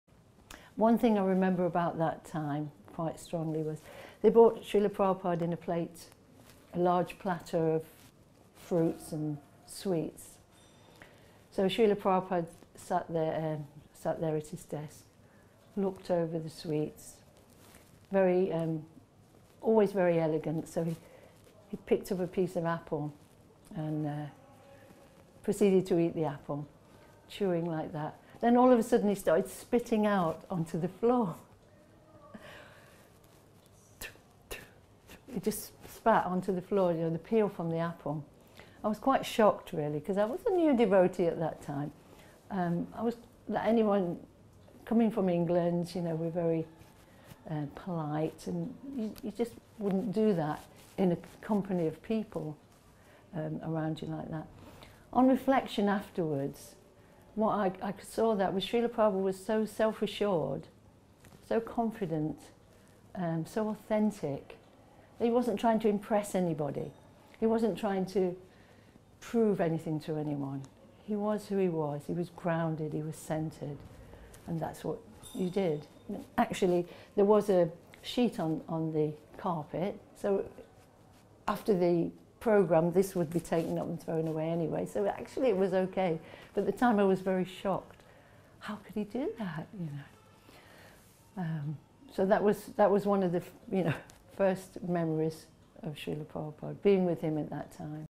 HG Janaki DD remembers Srila Prabhupada
Srila Prabhupada was authentic and not to impress anyone